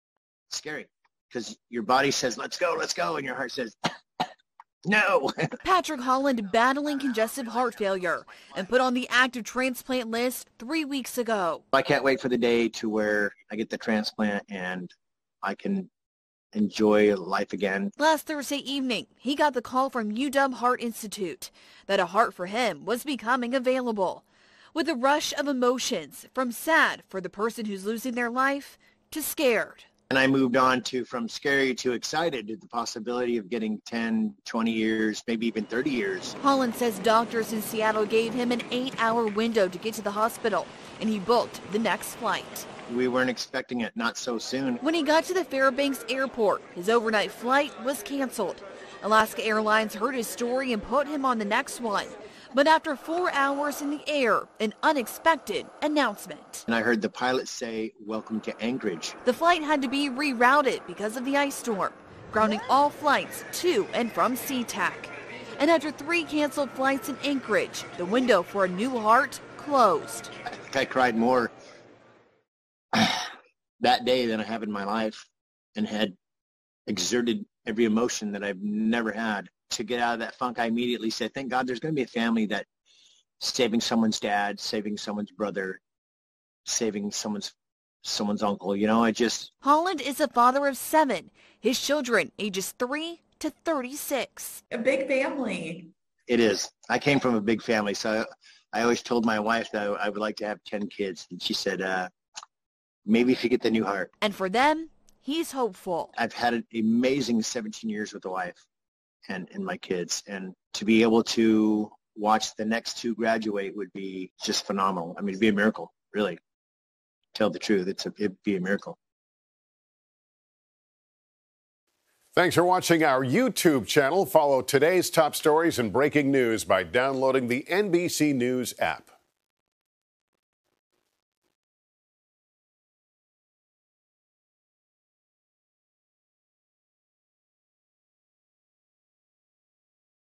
Alaska Man Misses Heart Transplant Due To Winter Storm
Patrick Holland is battling congestive heart failure and needed to catch a flight from Fairbanks, Alaska, to Seattle for a transplant, but due to the multiple flight cancellations at Sea-Tac Airport, Holland's heart went to another person. KING's Julie Calhoun reports.